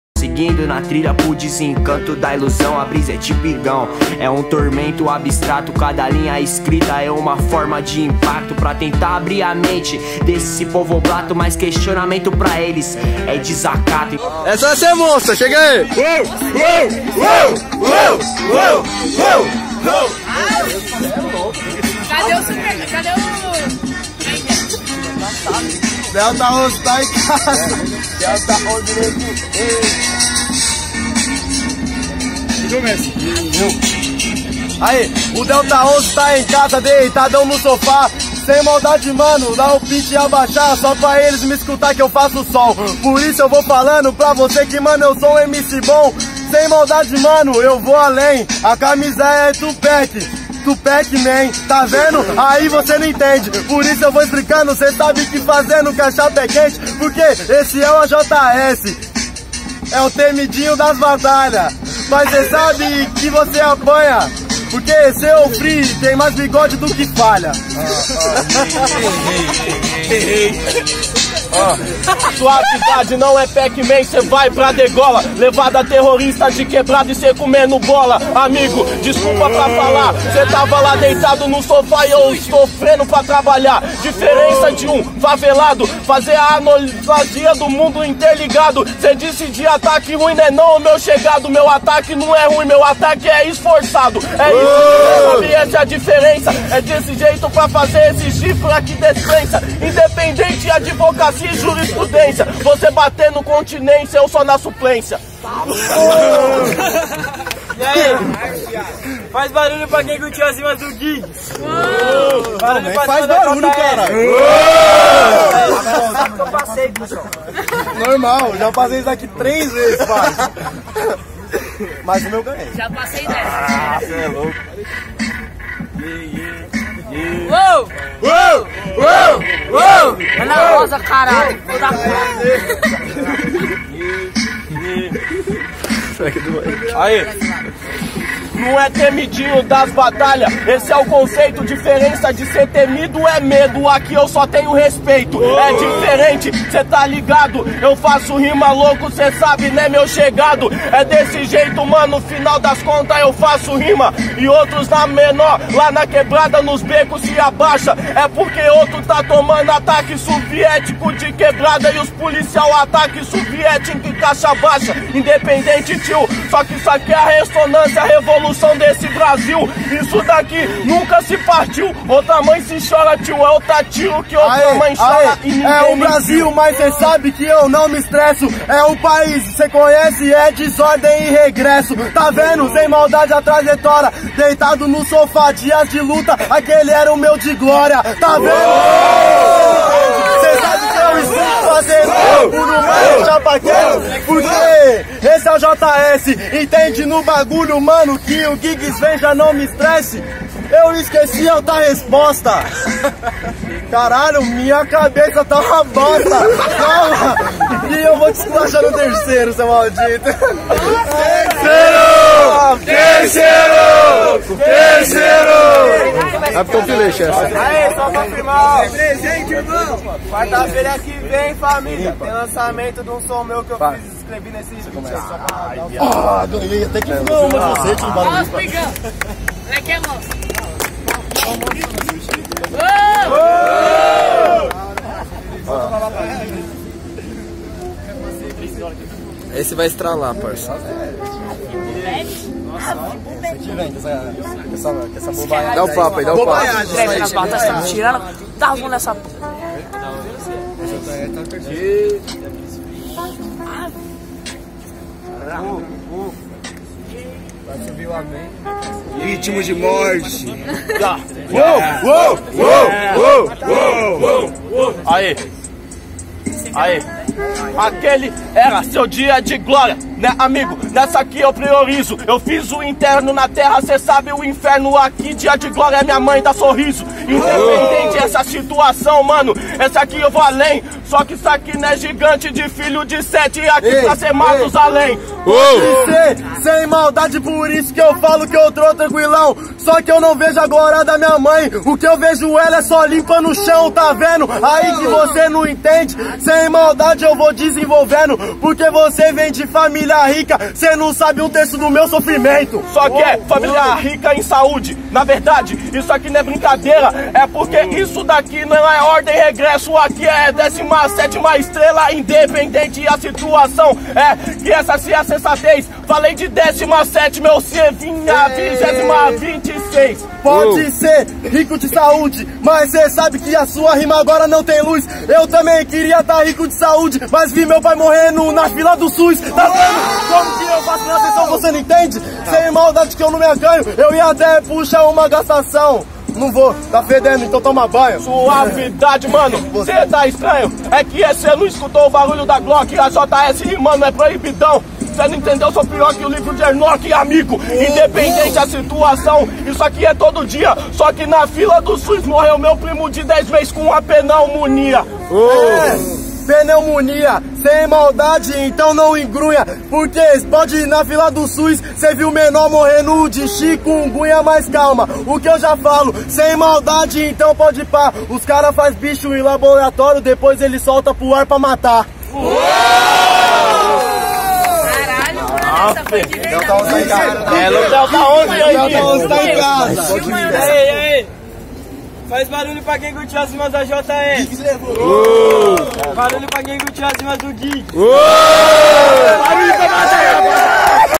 Guigs x AJS - 2FASE - 41° Batalha do Ana Rosa
Toda quarta-feira na Praça ao lado da saída do terminal de ônibus da estação Ana Rosa
Inicio da batalha: 19:00
Recomenda-se chegar antes para a entrega do nome e melhor organização.